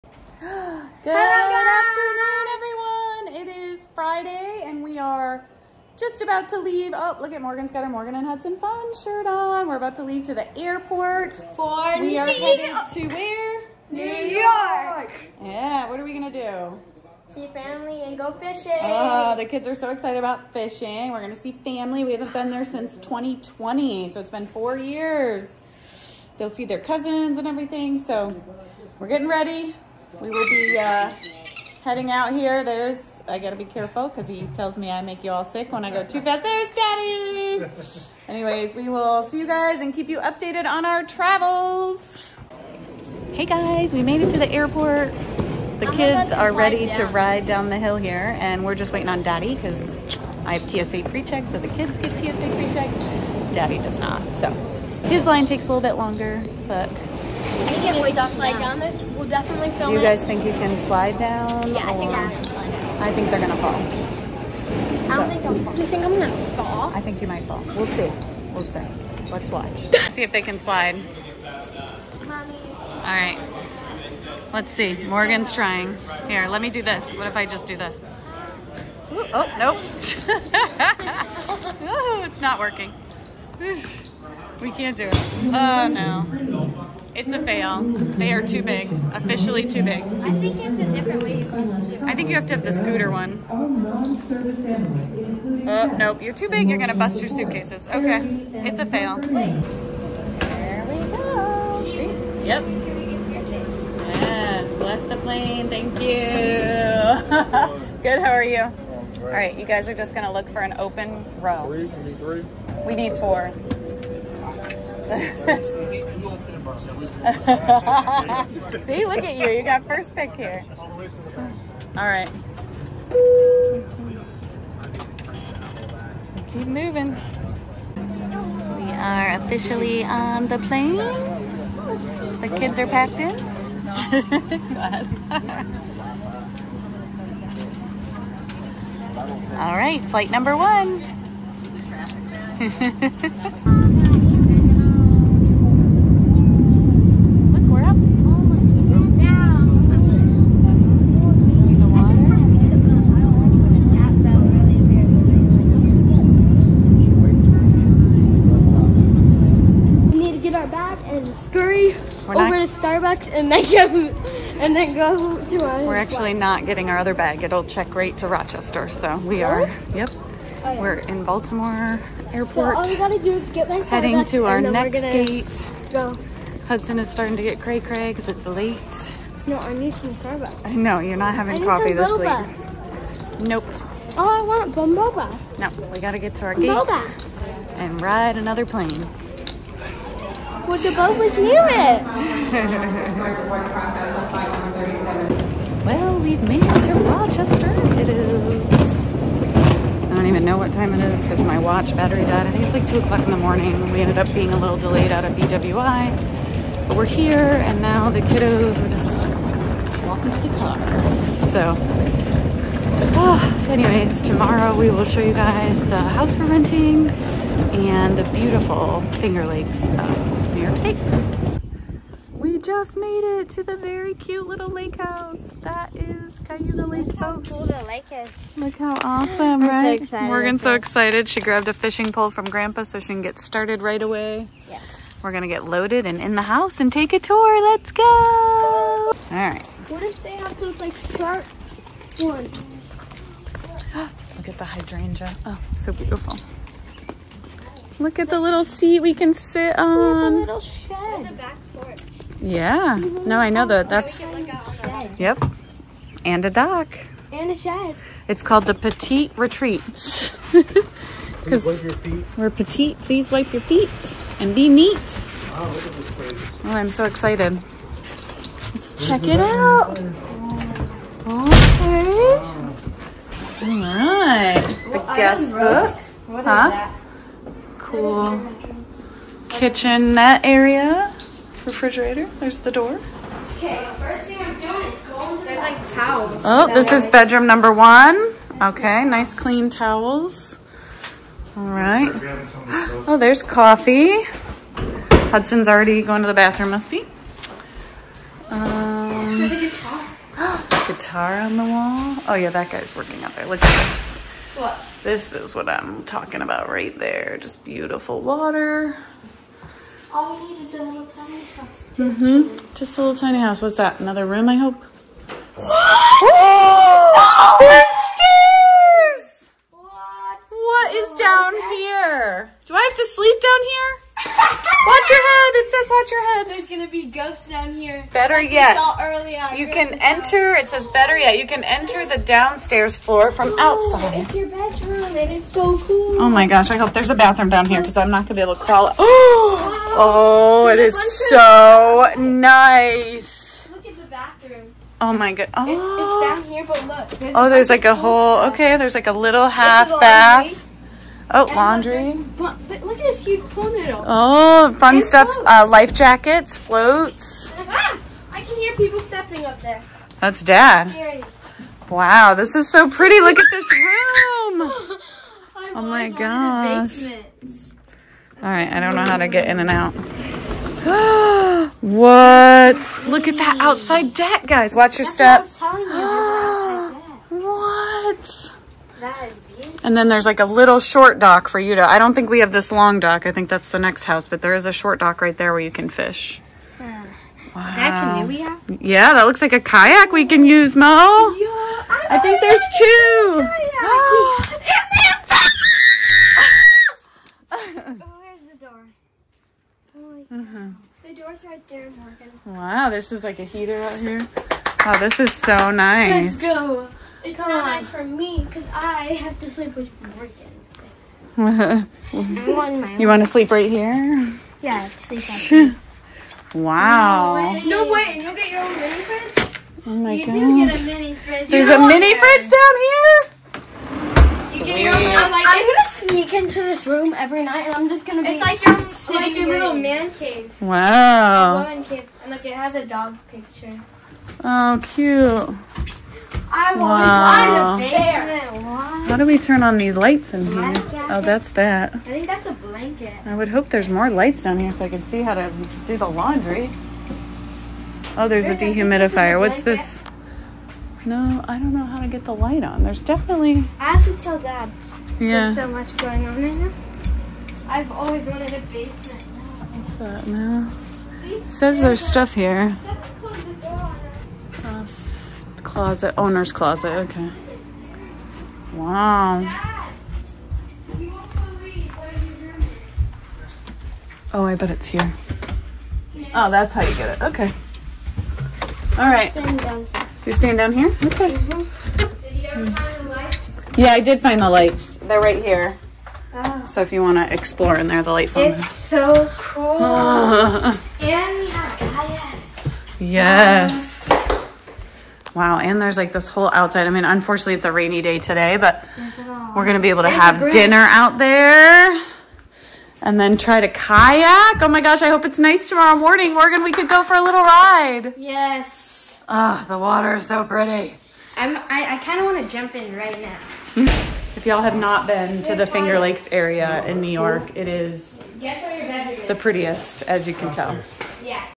Off to Upstate New York!!
We're off to NY to visit family, but first we have to get there!! We take to flights and EVENTUALLY make it to New York. There we give a tour of the house and a glimpse of what the Fingerlakes Region is known for. THE LAKES!! :)